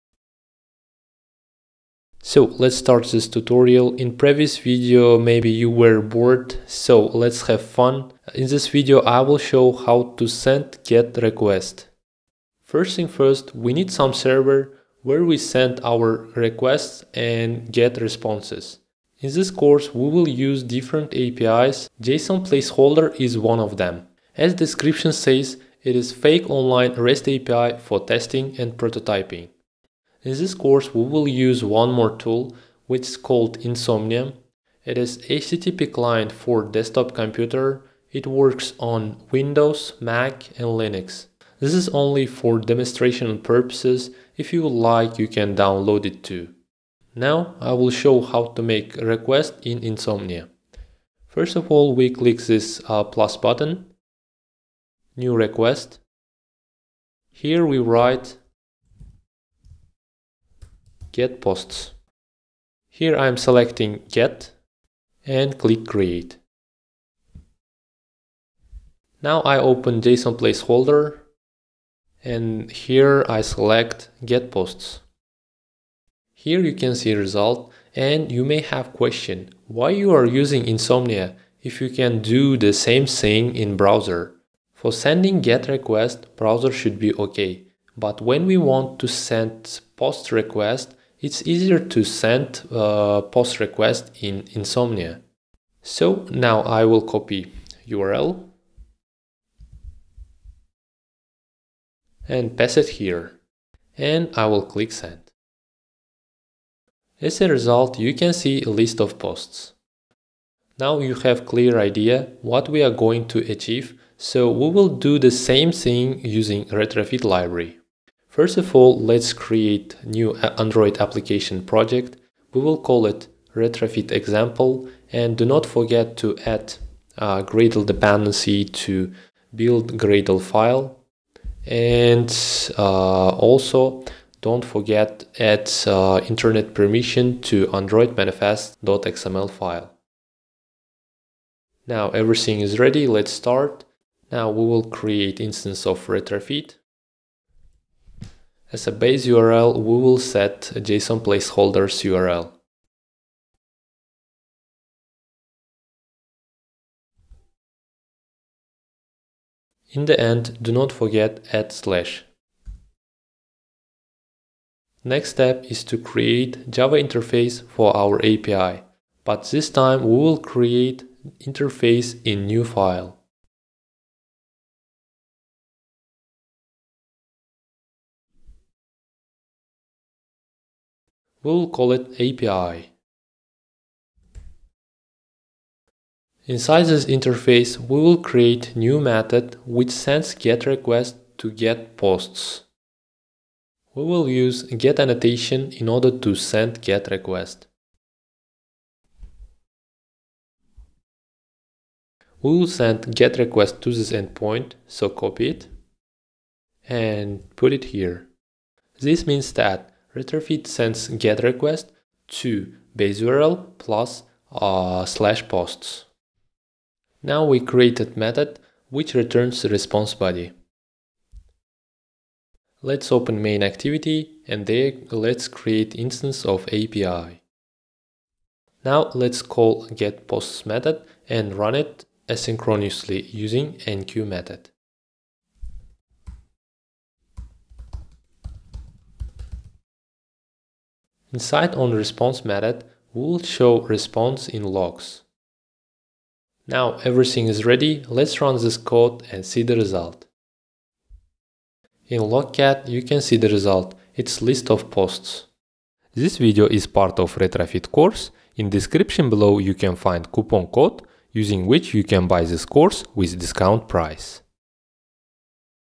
How to send GET request using Retrofit 2?
This video is part of 'Networking on Android using Retrofit 2' course. In this video I will explain how to send GET request with Retrofit 2